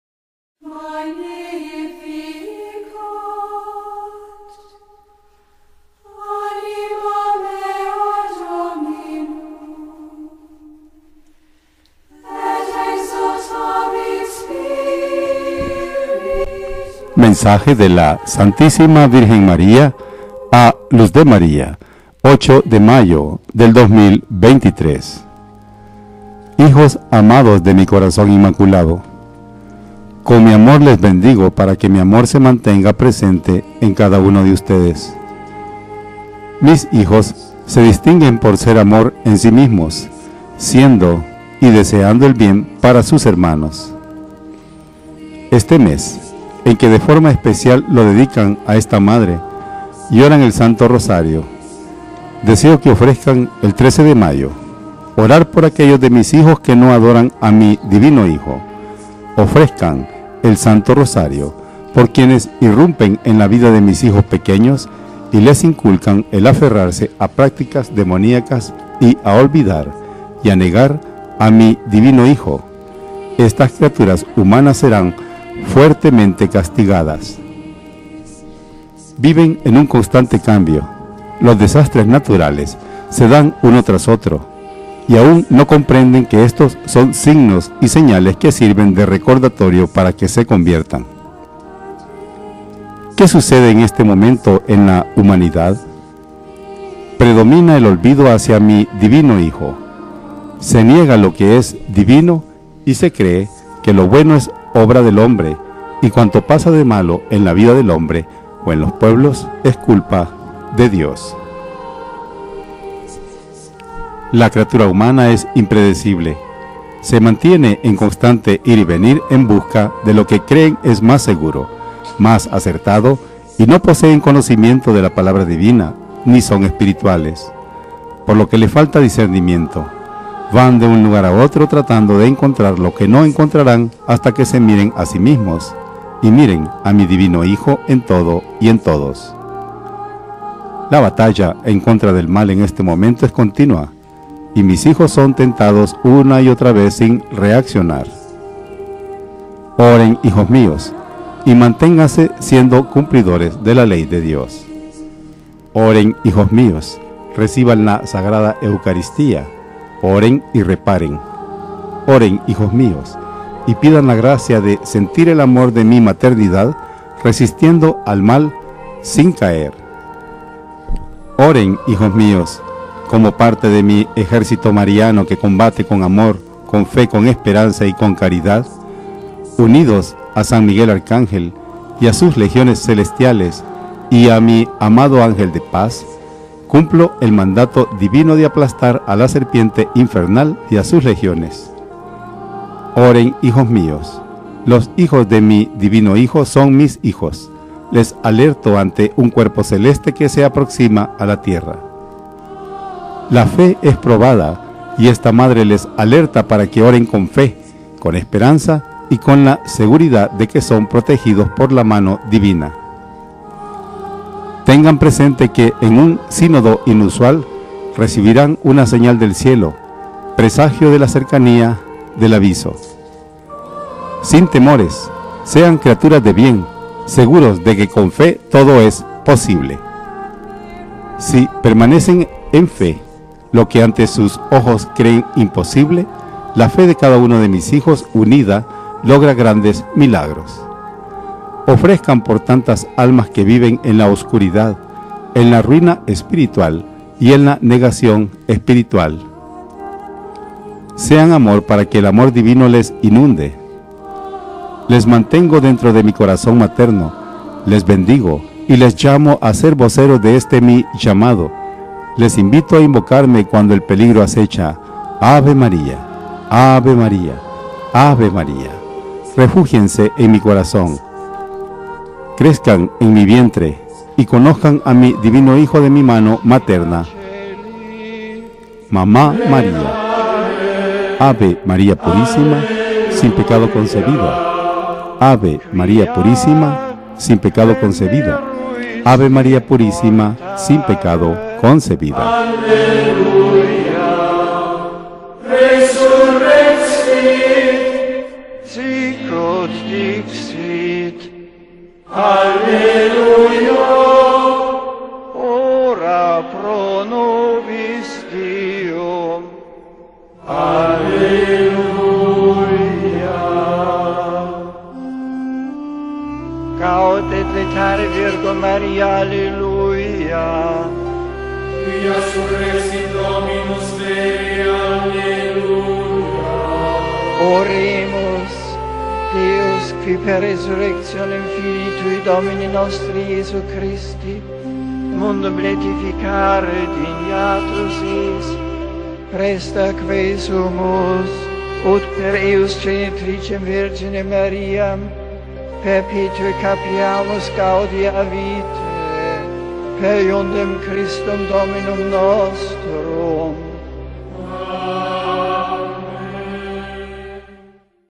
VIVEN EN UN CONSTANTE CAMBIO-LA FE ES PROBADA: Mensaje Santísima Virgen María a Luz de María 08/5/23
MENSAJE DE LA
SANTÍSIMA VIRGEN MARÍA
A LUZ DE MARÍA
08/05/23

Hijos amados de Mi Corazón Inmaculado:

CON MI AMOR LES BENDIGO PARA QUE MI AMOR SE MANTENGA PRESENTE EN CADA UNO DE USTEDES.  Mis hijos se distinguen por ser amor en sí mismos, siendo y deseando el bien para sus hermanos (Cf. I Jn. 4, 7-8).

Este mes en que de forma especial lo dedican a esta Madre y oran el Santo Rosario, deseo que ofrezcan el 13 de mayo:

Orar por aquellos de Mis hijos que no adoran a Mi Divino Hijo.
Ofrezcan el Santo Rosario por quienes irrumpen en la vida de Mis hijos pequeños y les inculcan el aferrarse a prácticas demoniacas y a olvidar y a negar a Mi Divino Hijo. Estas criaturas humanas serán fuertemente castigadas.

VIVEN EN UN CONSTANTE CAMBIO. Los desastres naturales se dan uno tras otro y aún no comprenden que estos son signos y señales que sirven de recordatorio para que se conviertan.

¿QUÉ SUCEDE EN ESTE MOMENTO EN LA HUMANIDAD?  Predomina el olvido hacia Mi Divino Hijo, se niega lo que es Divino y se cree que lo bueno es obra del hombre y cuanto pasa de malo en la vida del hombre o en los pueblos es culpa de Dios (Cf. Sant. 1, 13).

La criatura humana es impredecible, se mantiene en constante ir y venir en busca de lo que creen es más seguro, más acertado y no poseen conocimiento de la Palabra Divina ni son espirituales; por lo que les falta discernimiento. Van de un lugar a otro tratando de encontrar lo que no encontrarán hasta que se miren a sí mismos y miren a Mi Divino Hijo en todo y en todos.

La batalla en contra del mal en este momento es continua y Mis hijos son tentados una y otra vez sin reaccionar.

Oren hijos Míos y manténgase siendo cumplidores de la Ley de Dios.

Oren hijos Míos, reciban la Sagrada Eucaristía, oren y reparen.

Oren hijos Míos y pidan la Gracia de sentir el Amor de Mi Maternidad resistiendo al mal sin caer.

Oren hijos Míos, como parte de Mi Ejército Mariano que combate con Amor, con Fe, con Esperanza y con Caridad, unidos a San Miguel Arcángel y a Sus Legiones Celestiales y a Mi amado Ángel de Paz, cumplo el Mandato Divino de aplastar a la serpiente infernal y a sus legiones.

Oren hijos Míos, los hijos de Mi Divino Hijo son Mis hijos, les alerto ante un Cuerpo Celeste que se aproxima a la Tierra.

LA FE ES PROBADA Y ESTA MADRE LES ALERTA PARA QUE OREN CON FE, CON ESPERANZA Y CON LA SEGURIDAD DE QUE SON PROTEGIDOS POR LA MANO DIVINA.

Tengan presente que en un sínodo inusual recibirán una señal del Cielo, presagio de la cercanía del Aviso.

SIN TEMORES, SEAN CRIATURAS DE BIEN, SEGUROS DE QUE CON FE TODO ES POSIBLE (Cf. I Jn. 5,4; Mt. 9, 21-22). Si permanecen en fe, lo que ante sus ojos creen imposible, la fe de cada uno de Mis hijos unida logra grandes milagros.

OFREZCAN POR TANTAS ALMAS QUE VIVEN EN LA OSCURIDAD, EN LA RUINA ESPIRITUAL Y EN LA NEGACIÓN ESPIRITUAL.

Sean amor para que el Amor Divino les inunde.

Les mantengo dentro de Mi Corazón Materno.  Les bendigo y les llamo a ser voceros de este Mi Llamado. Les invito a invocarme cuando el peligro asecha:

Ave María, Ave María, Ave María.

Refúgiense en Mi Corazón, crezcan en Mi Vientre y conozcan a Mi Divino Hijo de Mi Mano Materna.

Mamá María

AVE MARÍA PURÍSIMA, SIN PECADO CONCEBIDA

COMENTARIO DE LUZ DE MARÍA

Hermanos:

En este llamado de Nuestra Santísima Madre sentimos ese Corazón que sin dudas late por y para Dios.  Esa criatura elegida, ese Vaso Sagrado que ante el saludo del Ángel pronunció: Fiat Voluntas Tua.

Hoy Nuestra Madre Santísima nos invita a acompañarla con esa fe que en las preocupaciones aumenta, con esa fe que cuando es perseguida se cubre con el escudo de la Preciosa Sangre de Cristo.

Hermanos, lo venidero no es fácil, pero no es imposible de mantenerse fiel a Cristo y a Nuestra Madre si el corazón y el pensamiento están colocados en la Voluntad Divina.

Se nos comparte una gran novedad:

El Ángel de Paz va a estar presente en la batalla final, unido a Nuestra Madre Santísima, a San Miguel Arcángel y a las Legiones Celestiales, se enfrentará al enemigo del alma y a sus seguidores.

Recordemos hermanos cuanto el Cielo nos ha revelado desde el año 2013 acerca del Ángel de Paz y abracemos desde ya tan infinita bendición reservada para esta generación y para el final de estos tiempos.  Les invito a meditar el libro donde se recopila la revelación  acerca del Ángel de Paz y a pedirle a Nuestra Madre nos acoja en Su Inmaculado Corazón y nos ayude a abrazar con verdadera confianza tan inmenso regalo del Cielo.

Hermanos, sin temor y con más fe que en el pasado, continuemos en Fe.

Amén.